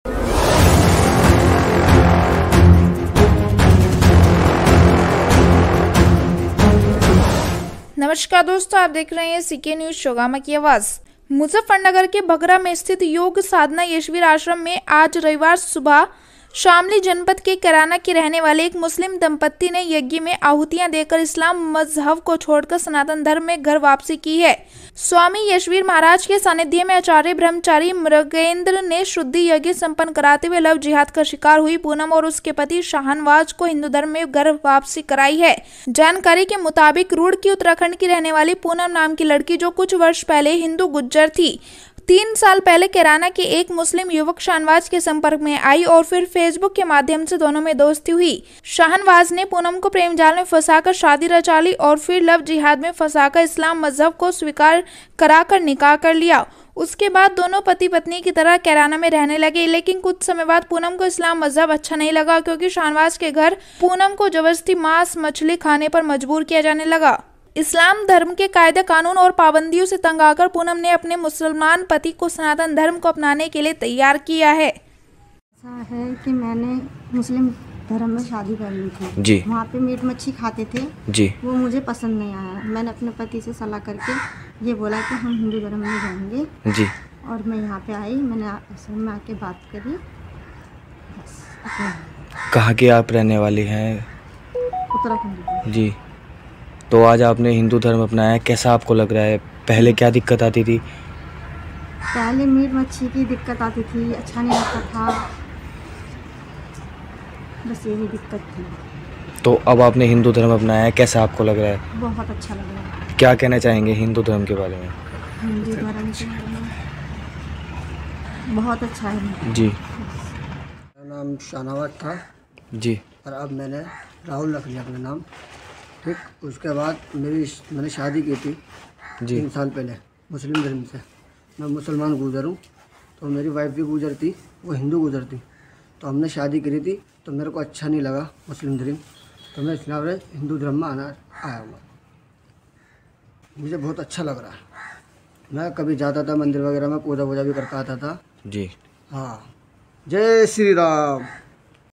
लव जिहाद का शिकार हुई पूनम ने मुस्लिम पति के साथ की घर वापसीyoutube
मुज़फ्फरनगर के बघरा में स्थित योग साधना यशवीर आश्रम में आज रविवार सुबह शामली जनपद के कैराना के रहने वाले एक मुस्लिम दंपत्ति ने यज्ञ में आहुतियां देकर इस्लाम मजहब को छोड़कर सनातन धर्म में घर वापसी की है। स्वामी यशवीर महराज के सानिध्य में आचार्य ब्रह्मचारी मृगेन्द्र ने शुद्धि यज्ञ संपन्न कराते हुए लव जिहाद का शिकार हुई पूनम और उसके पति शाहनवाज को हिन्दू धर्म में घर वापसी कराई है। जानकारी के मुताबिक रूडकी उत्तराखंड की रहने वाली पूनम नाम की लडकी जो कुछ वर्ष पहले हिन्दू गुज्जर थी तीन साल पहले कैराना के एक मुस्लिम युवक शाहनवाज के संपर्क में आई और फिर फेसबुक के माध्यम से दोनों में दोस्ती हुई। शाहनवाज ने पूनम को प्रेम जाल में फंसा कर शादी रचा ली और फिर लव जेहाद में फंसाकर इस्लाम मजहब को स्वीकार कराकर निकाह कर लिया। उसके बाद दोनों पति पत्नी की तरह कैराना में रहने लगे। लेकिन कुछ समय बाद पूनम को इस्लाम मजहब अच्छा नहीं लगा क्योंकि शाहनवाज के घर पूनम को जबरदस्ती मास मछली खाने पर मजबूर किया जाने लगा। इस्लाम धर्म के कायदे कानून और पाबंदियों से तंग आकर पूनम ने अपने मुसलमान पति को सनातन धर्म को अपनाने के लिए तैयार किया। अपनी प्रेमिका पत्नी की बात मानकर शाहनवाज और पूनम दोनो पति पत्नि ने योग साधना यशवीर आश्रम बघरा में यज्ञ में आहुतियां देकर इस्लाम मजहब को छोड हिन्दू धर्म में घर वापसी कर ली है। बघरा योग साधना केंद्र के संस्थापक स्वामी यशवीर जी महाराज ने इस मुस्लिम दंपत्ति को पूरी विधि विधान पूर्वक सनातन धर्म की दीक्षा देकर सनातन धर्म में घर वापसी कराई। स्वामी यशवीर महराज ने इनको गले में पटका व देवी देवताओं के चित्र देकर सम्मानित किया और गंगा जल से आचमन कराकर यज्ञोपवीत धारण करा संकल्प कराया कि जीवन के आखिरी सांस तक सनातन धर्म में रहकर सनातन धर्म की सेवा करेंगे और अपने जीवन को समाज सेवा में लगाते हुए आध्यात्मिक जीवन जीयेंगे। ब्रहाचारी मृगेन्द्र ने दोनों को अयोध्या की मिट्टी का तिलक लगाकर अभिनंदन किया। शाहनवाज का नाम राहुल रखा गया और अर्शी को पहला हिन्दू नाम पूनम दिया गया। योग साधना यशवीर आश्रम में दो वर्ष में लगभग 1200 मुसलमानों ने इस्लाम मजहब को छोड़कर सनातन धर्म में घर वापसी की है।